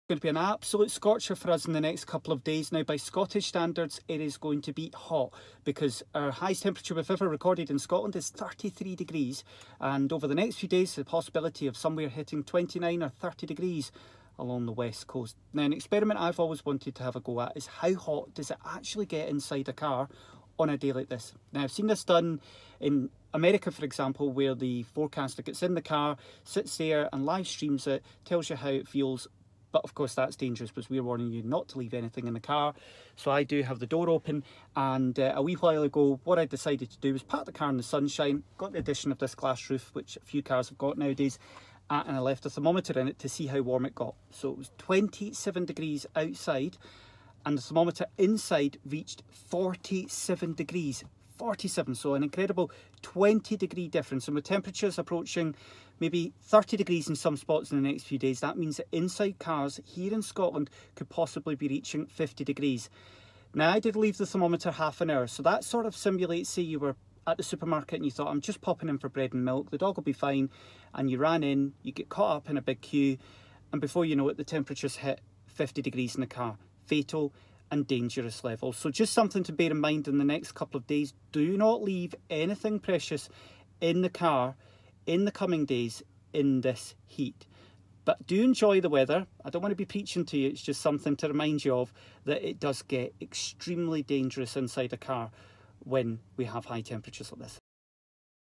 Sean Batty urges Scots to not leave dogs in hot cars
STV Weather's Sean Batty is urging Scots not to leave dogs in cars during high temperatures after conducting an experiment.

The meteorologist left a thermometer in a car on a hot day and after 30 minutes, the temperature read 47C.

Sean's warning comes as temperatures are set to reach up to 30C on the west coast of Scotland in the coming days.